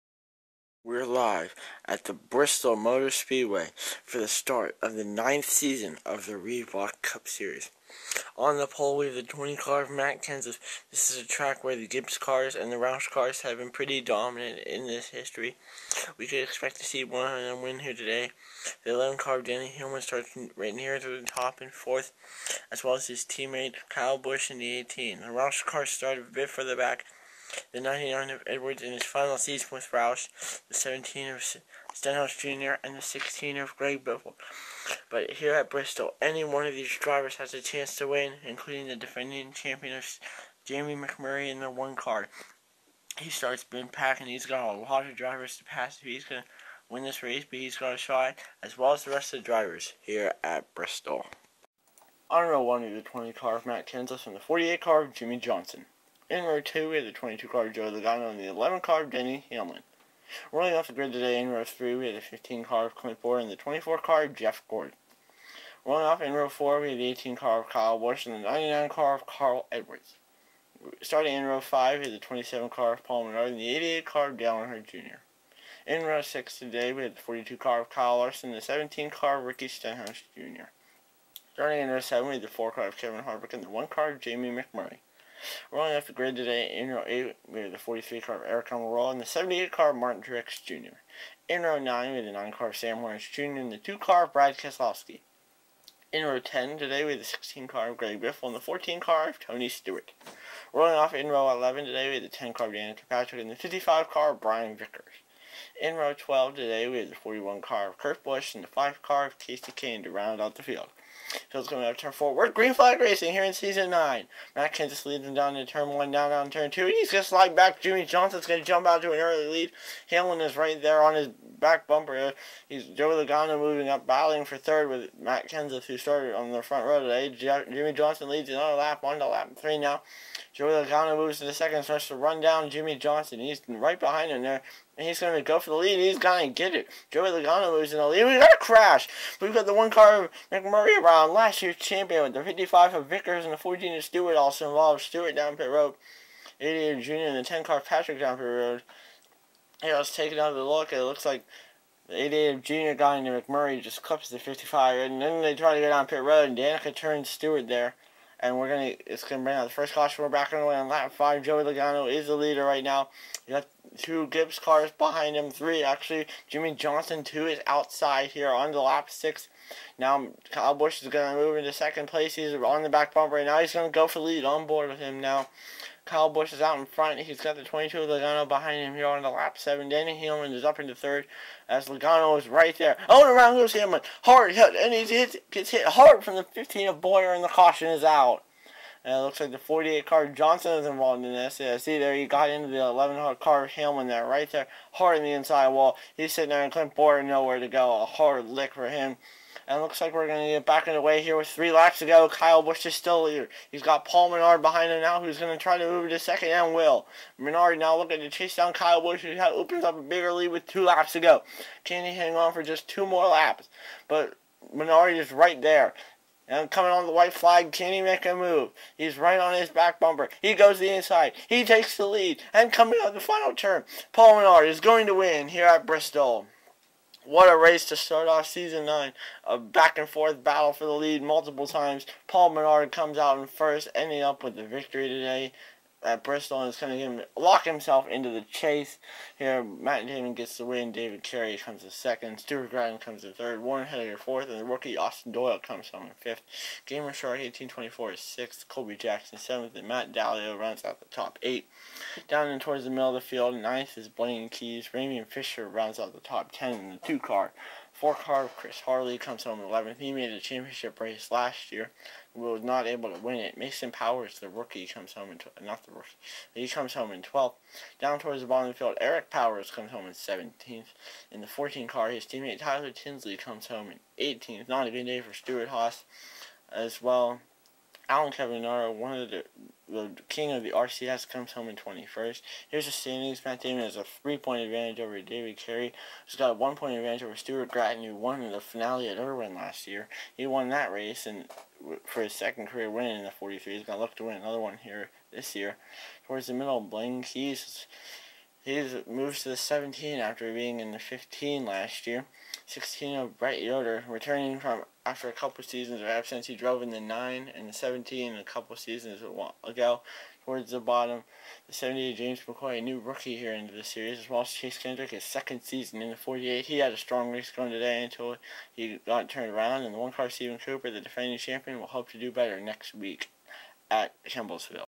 Reebok Cup Series Season 9 Race 1 Bristol
Season 9 begins in the Appalachian Mountains at the small half mile of Bristol!

(If you are in the series, please comment!)

Race 2 from Kemblesville!